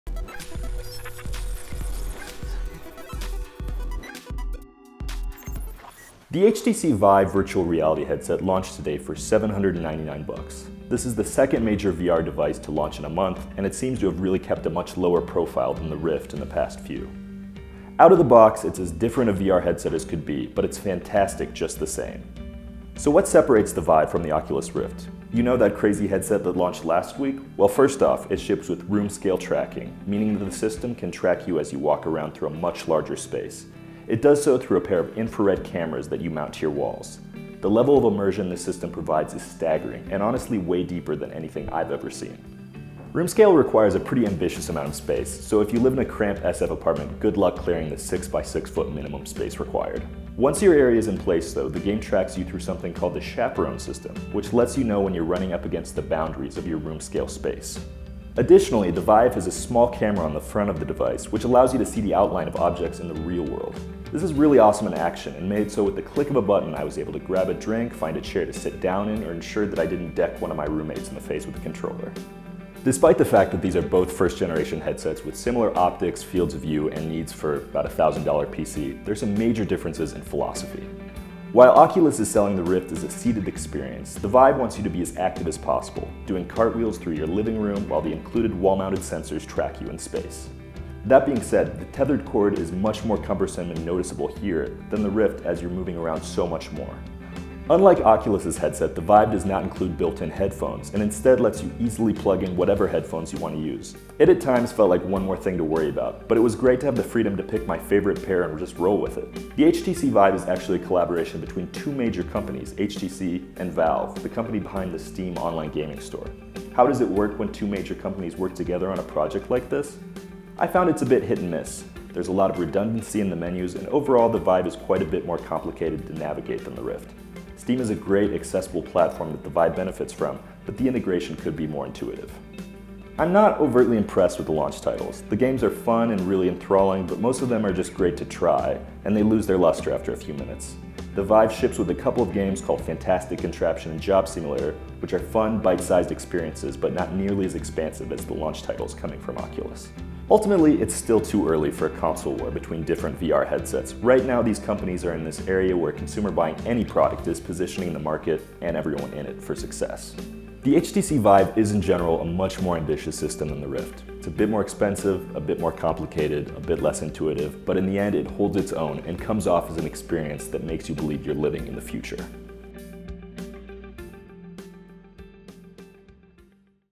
HTC Vive Review
The HTC Vive virtual reality headset launches today for $799. The level of immersion this system provides is staggering thanks to a pair of infrared cameras that you mount to your walls. Additionally, the Vive has a small camera on the front of the device which allows you to see the outline of objects in the real world. Lucas Matney demos the HTC Vive.